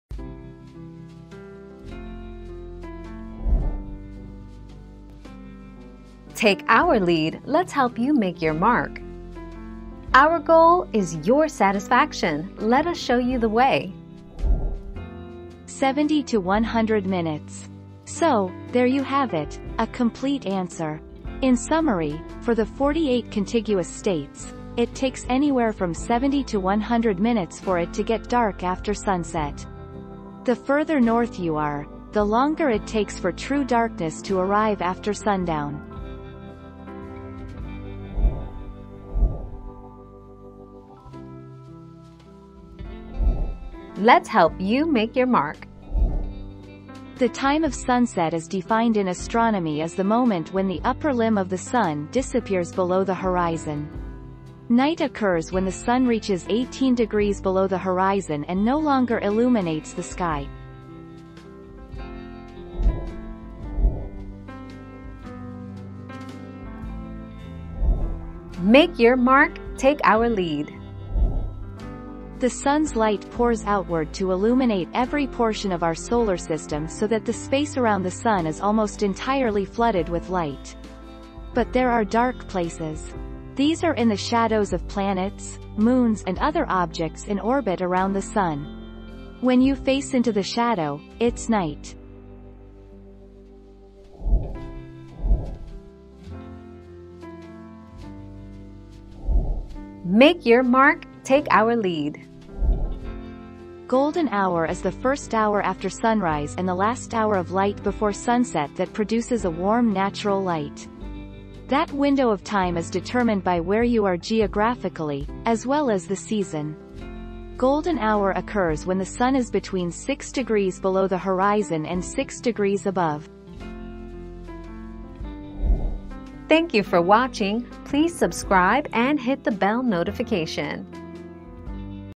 How long does it take for it to be dark after sunset?
00:00 - How long does it take for it to be dark after sunset?
00:40 - Does sunset time mean dark?
01:04 - How does it get dark?
01:35 - How long before sunset is golden hour?

Laura S. Harris (2021, April 6.) How long does it take for it to be dark after sunset?
    AskAbout.video/articles/How-long-does-it-take-for-it-to-be-dark-after-sunset-208393